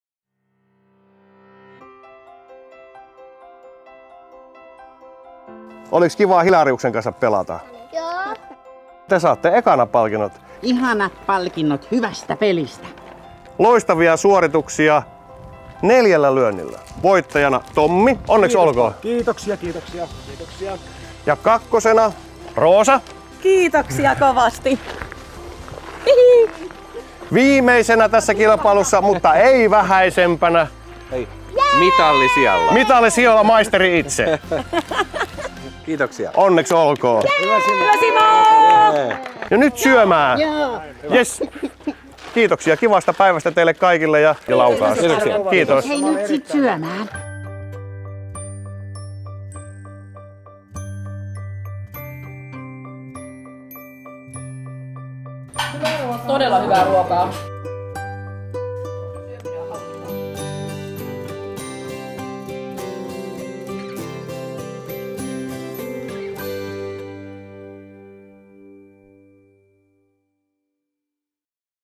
Palkintojen jako.
Upea golfpäivä huipentuu palkintojen jakoon ja juhlalliseen illalliseen.